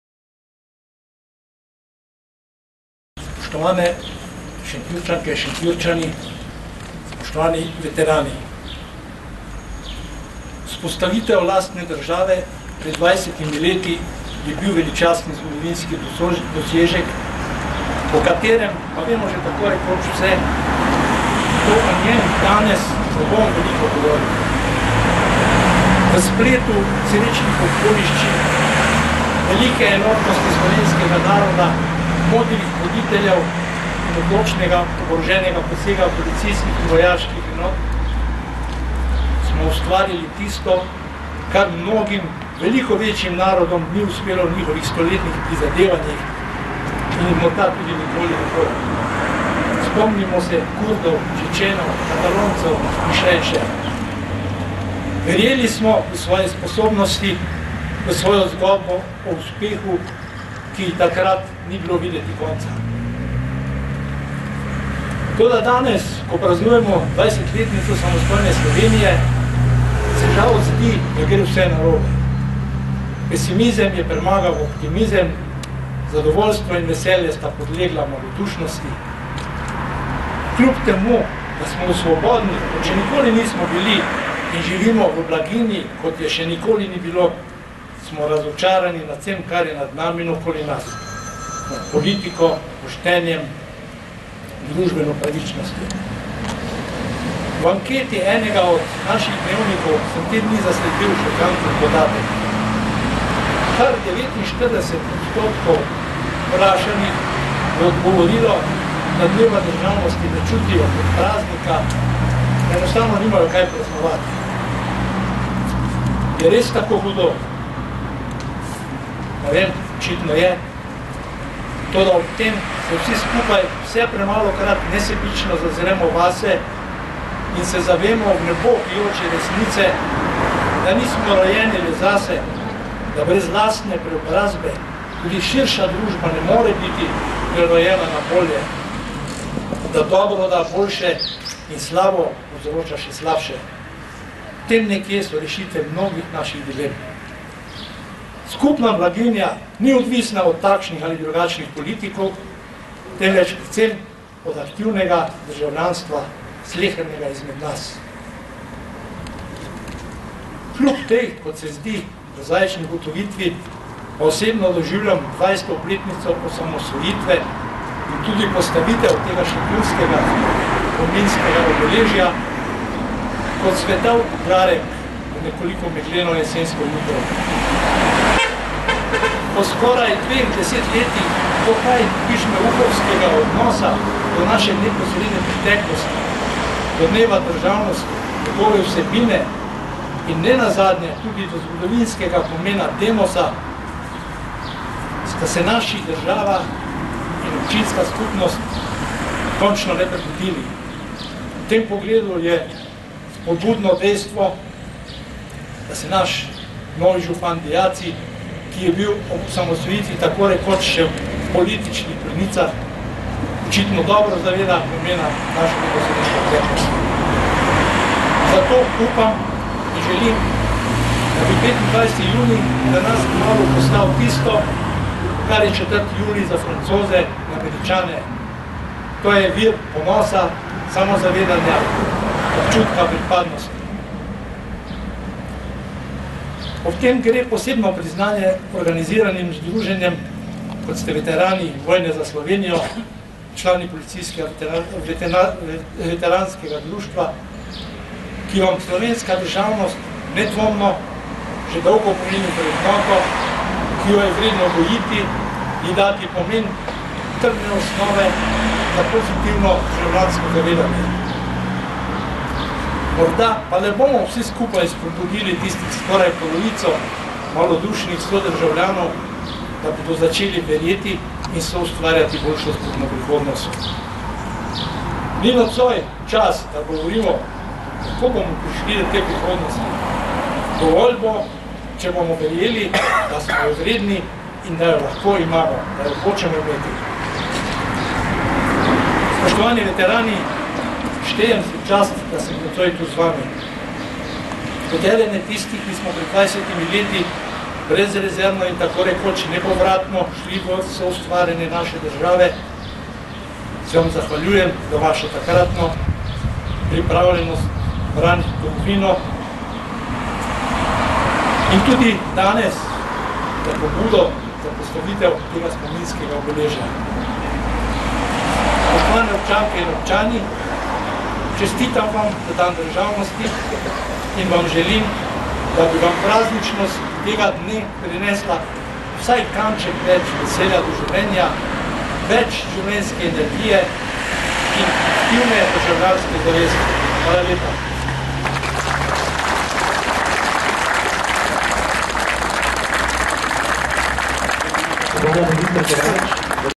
Mag. Franc Kovač
Nagovor predsednika Skupščine občine Šentjur pri Celju leta 1991 ob dnevu državnosti ob 20. obletnici osamosvojitve Republike Slovenije na Mestnem trgu v Šentjurju